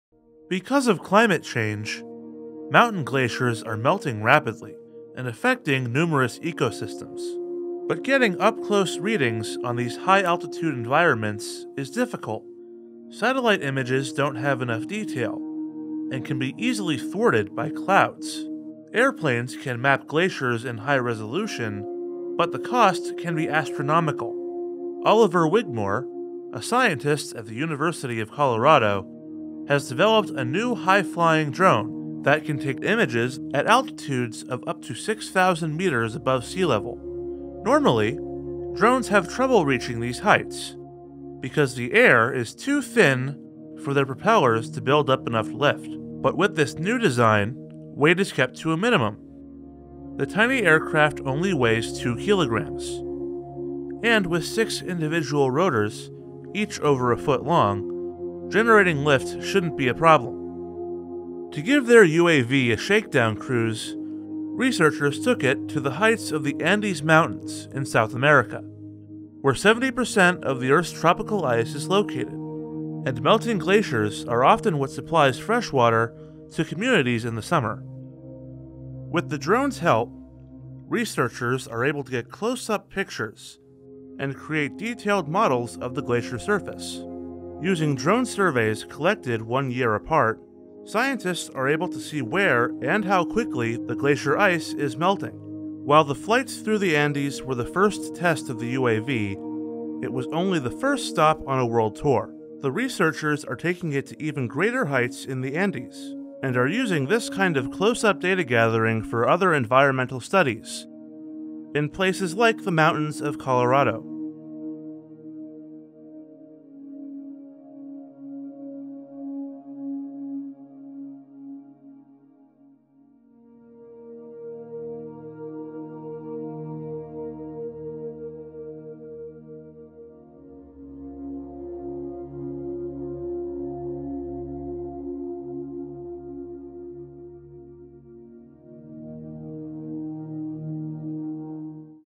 This high-flying drone can soar up to 6000 meters above sea level
Custom-built research copter gets better images of Peruvian Andes ice than satellites.